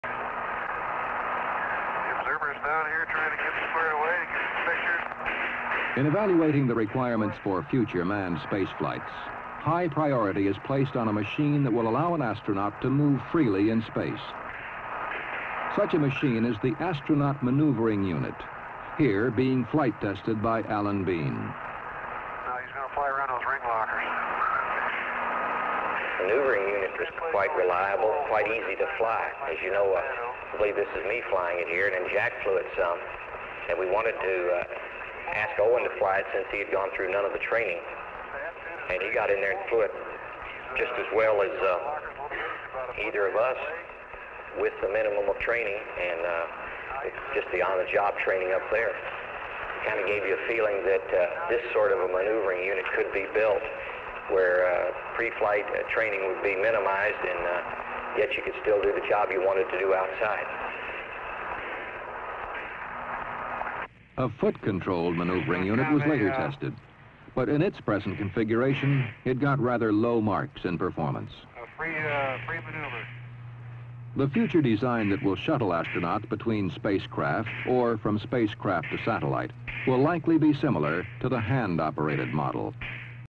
Astronaut Propulsion Unit -- Early Prototypes of MMU on Skylab
Astronaut Propulsion Unit -- Early Prototypes of MMU on Skylab. From Wikipedia:  "The Manned Maneuvering Unit (MMU) is an astronaut propulsion unit which was used by NASA on three space shuttle missions in 1984. The MMU allowed the astronauts to perform untethered EVA spacewalks at a distance from the shuttle. The MMU was used in practice to retrieve a pair of faulty communications satellites, Westar VI and Palapa B2. Following the third mission the unit was retired from use. A smaller successor, the Simplified Aid for EVA Rescue (SAFER), was first flown in 1994, and is intended for emergency use only."